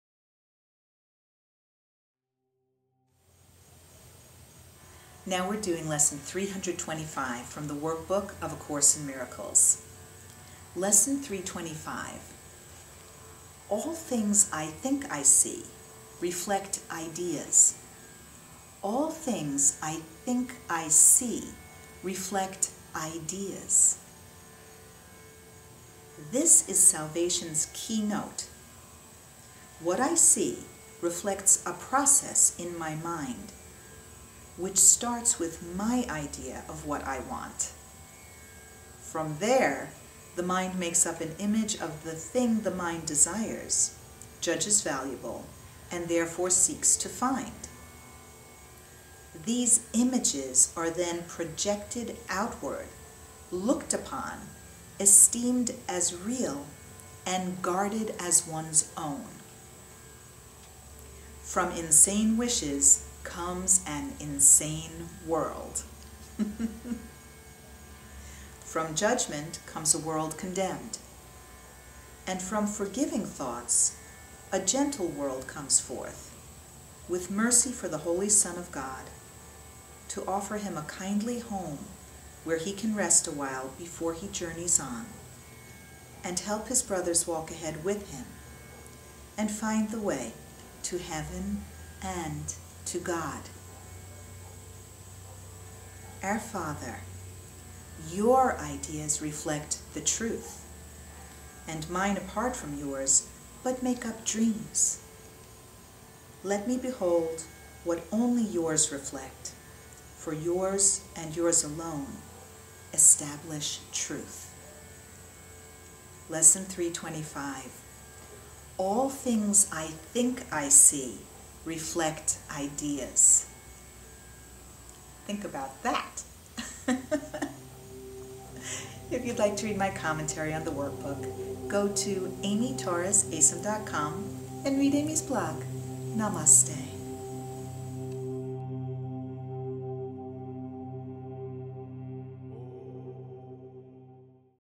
Workbook Lesson 325 from A Course in Miracles with Amy Torres
Lesson 325:  All things I think I see reflect ideas.